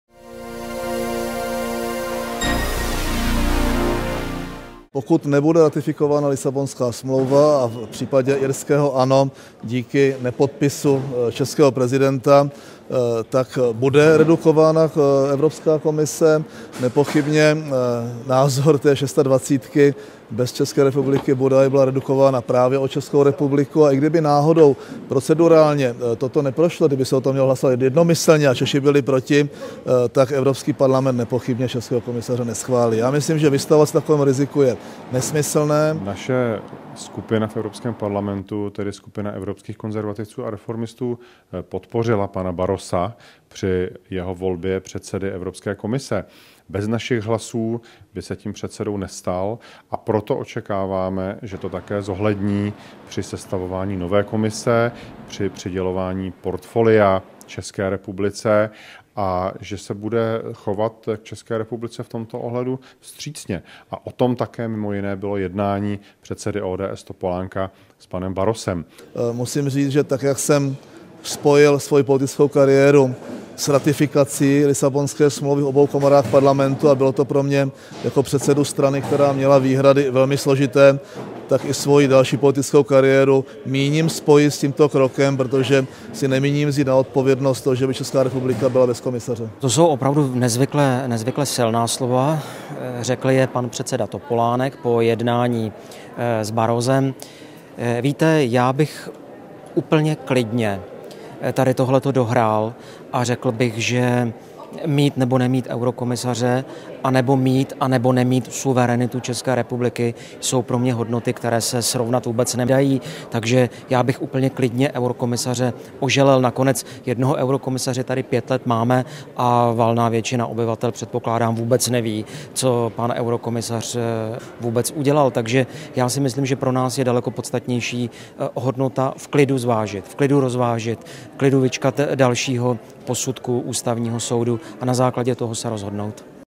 Reportáž: Osud Lisabonské smlouvy rozhodne o eurokomisaři
- Předseda ODS Mirek Topolánek, europoslanec Jan Zahradil a europoslanec Ivo Strejček se vyjadřují k osudu Lisabonské smlouvy, která stále čeká na podpis prezidenta Václava Klause. Do všeho se ale vložili senátoři, kteří chtějí po Ústavním soudu přezkum některých ustanovení.